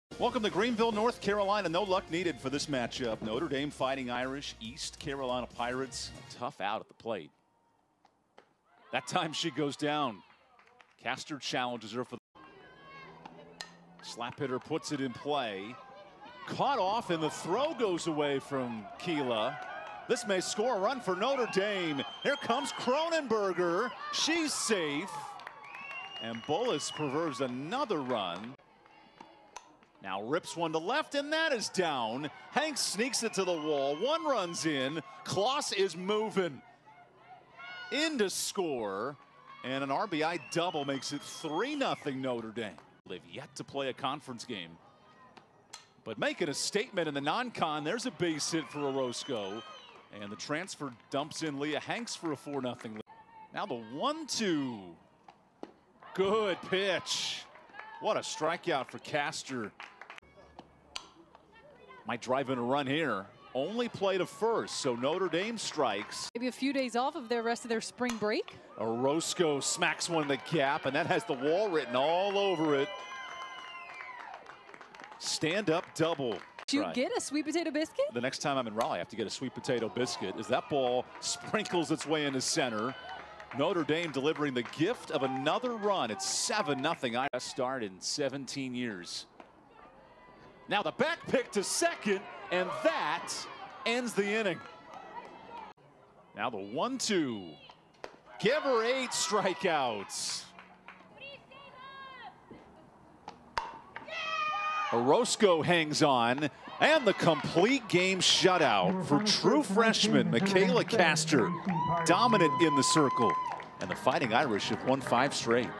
Freshman Pitches Complete Game Shut Out | Highlights vs. East Carolina | Notre Dame Softball
The University of Notre Dame softball team closed its spring break trip in North Carolina with a 7-0 victory over the East Carolina Pirates Thursday evening at Max R. Joyner Family Stadium. The Fighting Irish hung crooked numbers in two innings, while freshman Micaela Kastor held ECU hitless through five to earn the 16th victory of the season.

Kastor started in the circle, throwing her second complete game, first 7.0 inning CG. The freshman struck out a season-high nine hitters in the contest, allowing just three hits and walking four.

The Irish offense was led by a pair of two hit efforts. Leea Hanks finished 2-for-4 with two RBI, two runs scored and a double. Lexi Orozco added a 2-for-3 performance with a double, two RBI and scored a run. Payton Tidd and Mickey Winchell each added an RBI to go with a hit. Miranda Johnson finished with a base knock. Carlli Kloss added two runs scored, reaching base with a walk and a hit by pitch.

After two scoreless innings, the Irish broke the tie in the top of the third. Jane Kronenberger was hit by a pitch to start the rally as Winchell reached on an infield hit. The ECU defense threw it away at first, and couldn’t pick it up clean, allowing Kronenberger to score from first on the play, and Winchell to take third. After Kloss was hit by a pitch, Hanks delivered a double to center field to score both and extend the lead to 3-0. A fly out to deep left moved Hanks up and Orozco brought her in with a single to take lead at 4-0.

After a scoreless fourth, Notre Dame added on three more in the fifth. A walk to Kloss started the rally as she went first to third on a single to right. Gaskins brought a run in with a ground out and Orozco doubled to bring in another. Tidd extended the lead with a single of her own to bring in the third run of the inning, extending the lead to 7-0.

ECU got runners on in the sixth and seventh, but Kastor worked out of the jams to preserve the 7-0 win.